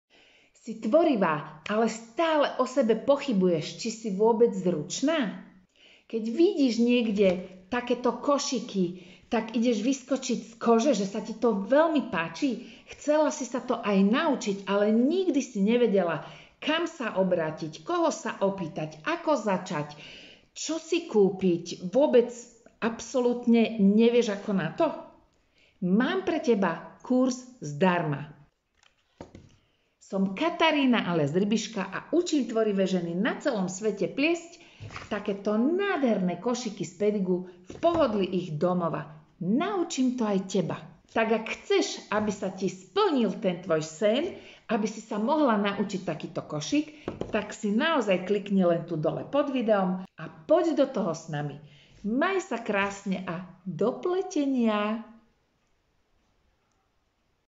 Si tvorivá, zručná, ale nevieš, či dokážeš upliesť košík?
❤️  Milí žena tvorivá, miluješ ručné práce a učarovalo ti pletenie košíkov? Aj keď si veľmi zručná, cez to všetko je pletenie pre teba trápenie a drina? Pletieš, skúšaš, ale ani pri ďalších košíkoch si stále:
❌ nevieš namerať správnu dĺžku osnov?
❌ nevieš si pripraviť správny mix materiálu?
❌ nevieš udržať rovné osnovy a košík je tak potom krivý?
❌ nejde ti tvarovanie košíka a stále je to pre teba španielska dedina?
❌ nevieš ako prúty nadpájať?
❌uzávierka je stále tvoj strašiak a vždy ju pletieš štýlom pokus-omyl s neistým výsledkom?